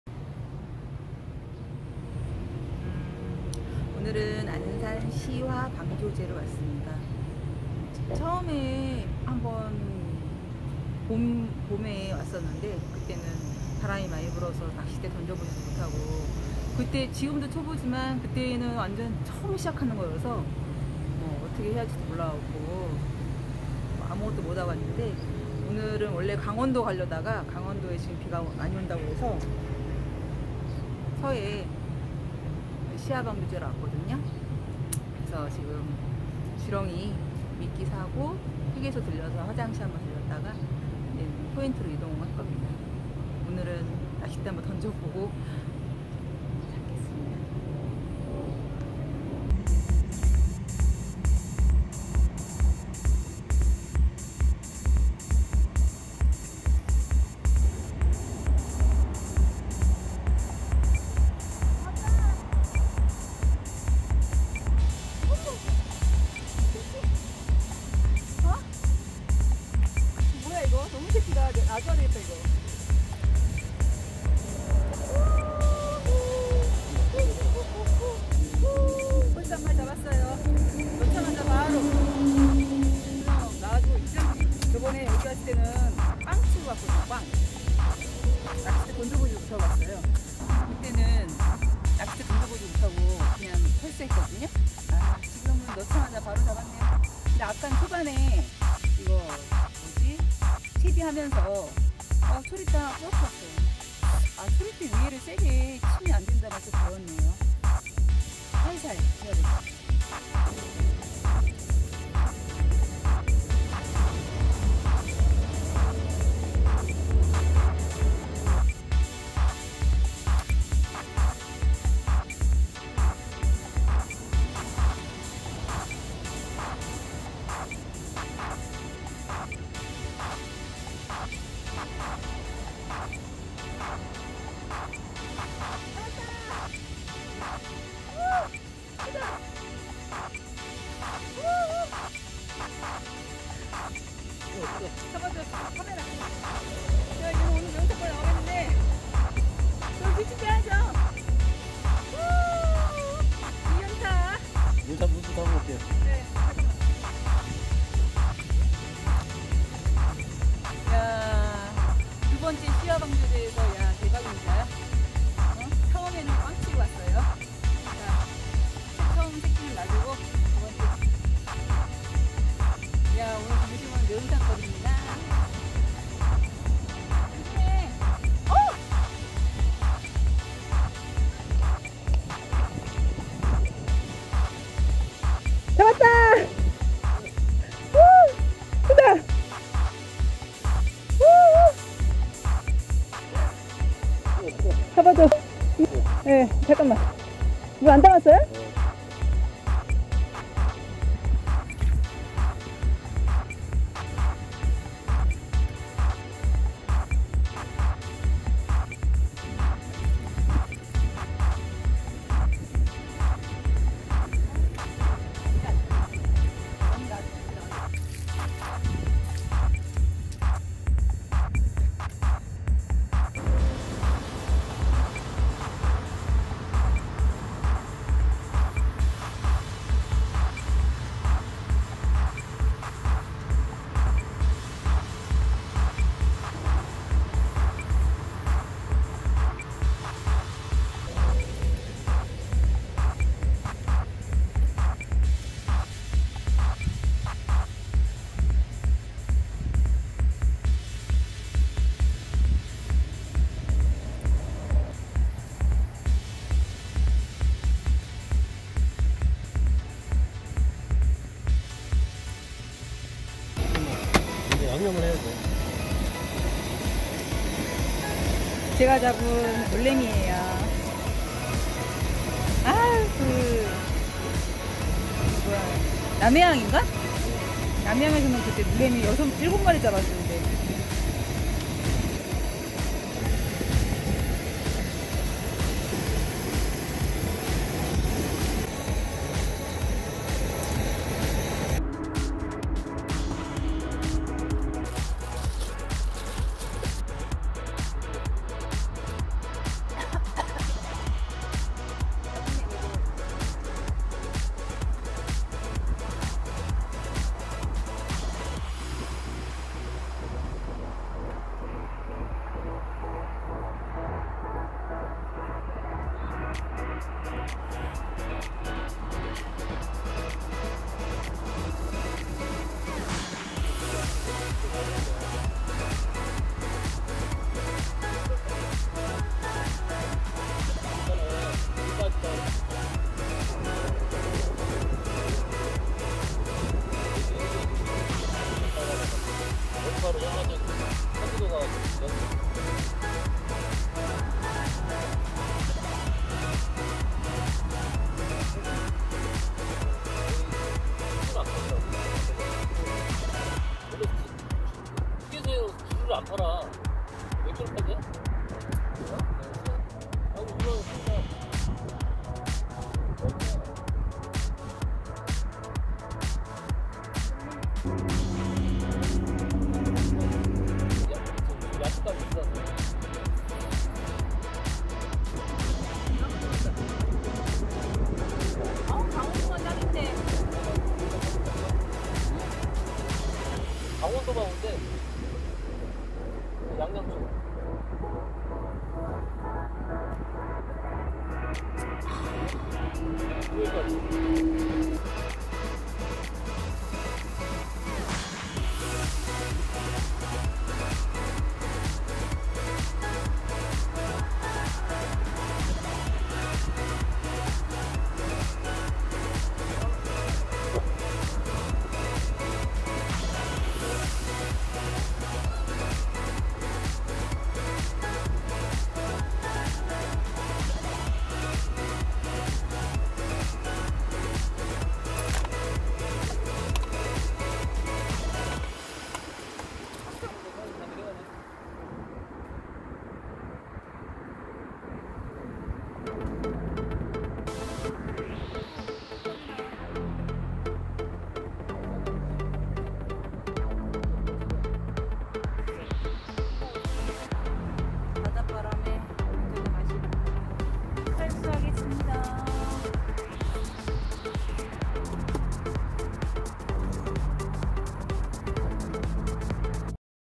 직접잡은 노래미 매운탕 끓여먹고.물고기도 잡고 .시원한맥주에 푸른 바다향기~
안녕하세요..

처음에는 낚시대 던져보지도 못하고 돌아갔지만 ~두번째는 캐스팅하자마자

물고기를 잡았어요^^ 서해라 물이 빠져 많이 잡지는 못했지만 처음 꽝친곳

에서 이렇게 노래미.우럭.등 다양한 어종을 잡아서 나름 기분이 좋습니다^^

조금씩 발전해가는 모습으로 찾아뵙겠습니다~